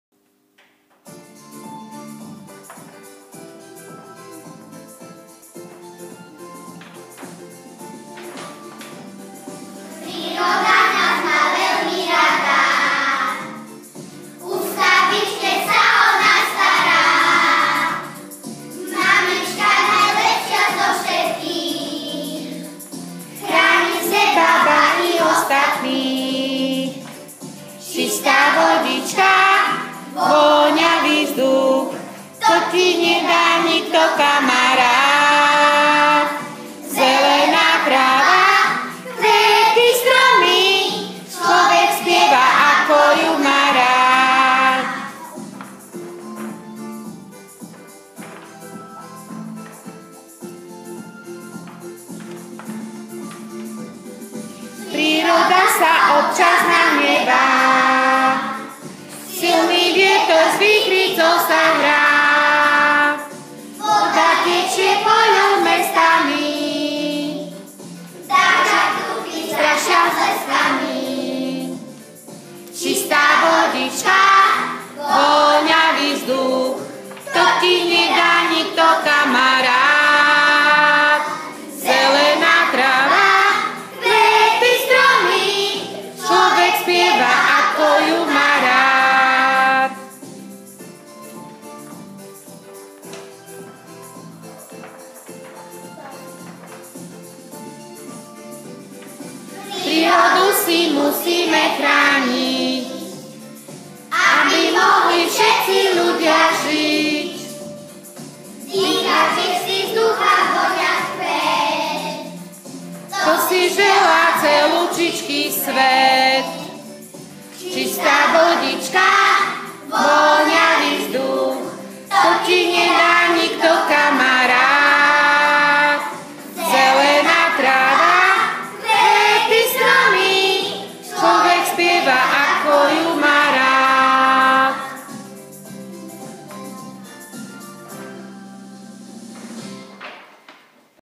Projekt EKOROK v 1.A
S ochranou prírody sa žiaci stretávajú v rôznych predmetoch. Deti z 1.A v rámci výtvarnej výchovy maľovali listnaté i ihličnaté stromy a odtlačkami dlaní a prstov im dávali prirodzený tvar. Na hodine prvouky vystrihovali z farebného papiera liečivé rastliny, o ktorých čítali v šlabikári. Nakoniec si ich nalepili na kartón.  Na hudobnej výchove sa naučili pieseň o prírode, jej veľkej sile, o potrebe si ju vážiť, chrániť aj obnovovať. Tu je video, ktoré vypovedá o projekte, do ktorého sa zapojili.